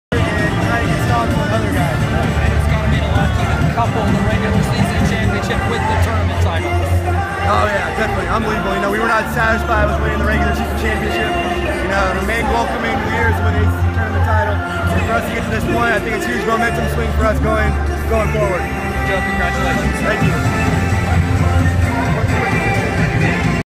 Joe Harris Post Victory
Harris talks to the press following Virginia's huge win.